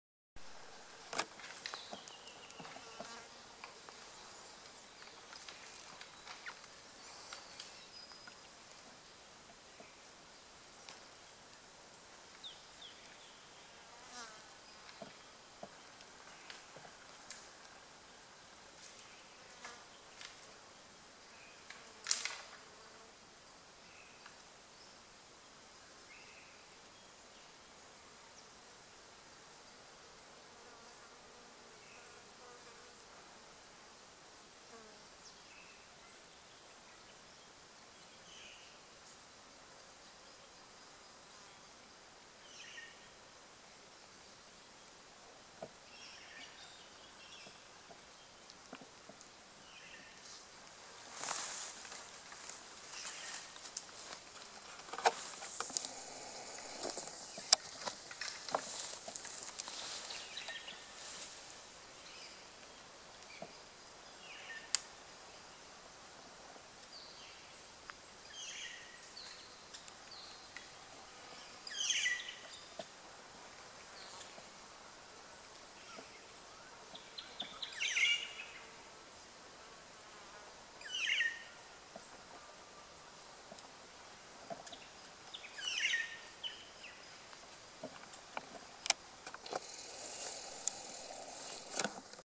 rubbing fallow
caught this fallow i named flame trees rubbing out when having a hunt with a good mate. we both filmed with buck and took some stills but as you can see the grass and light made hard work for great stills even being 25 yards from him